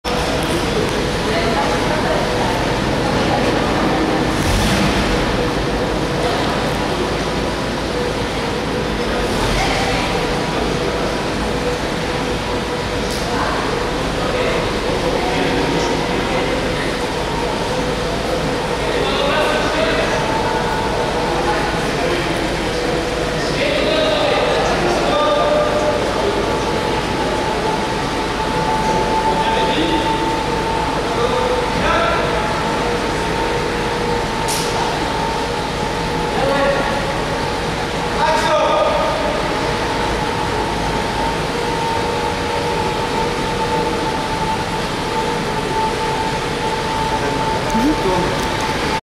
Filmmaking scene at Paris-Winners 2011
Ready, Action! On the way back to the hotel after our trip to Normandy we saw a crew shooting a film in the tunnel under our hotel. I guess it must have been an action movie and we may be guest starring it!